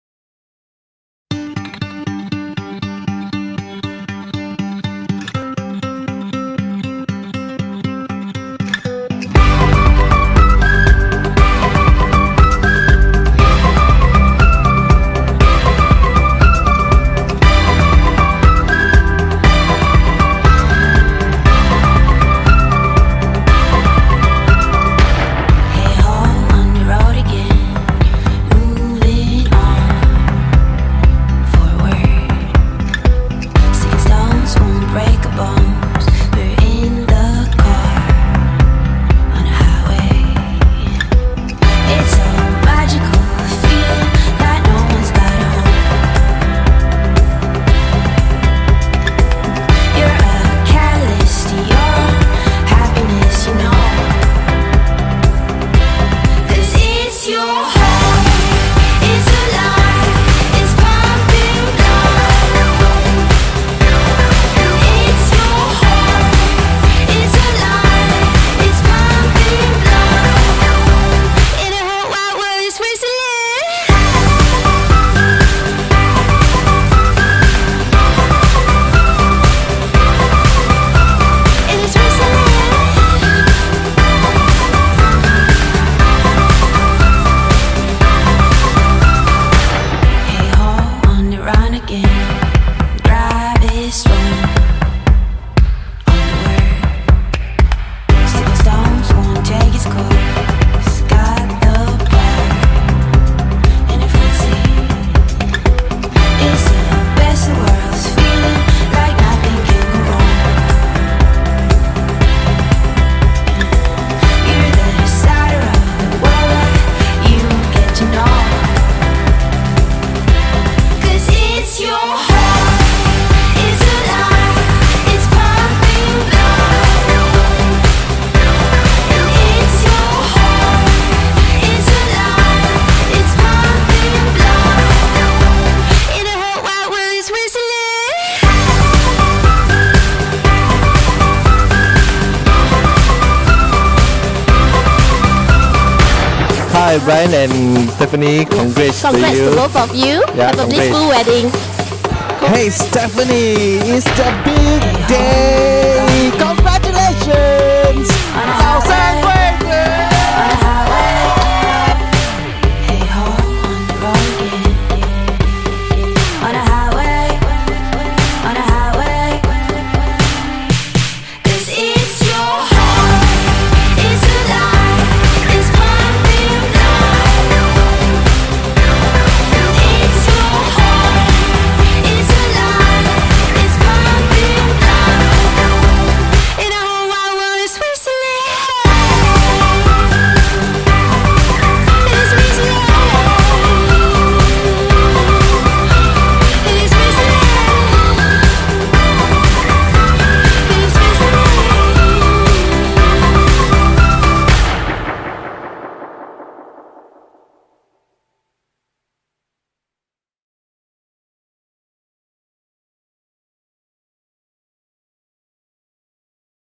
GC Masterpiece Presents | Stephanie and Ryan's Wedding
Slow Motion Highlight Reel with Gold Fringe Backdrop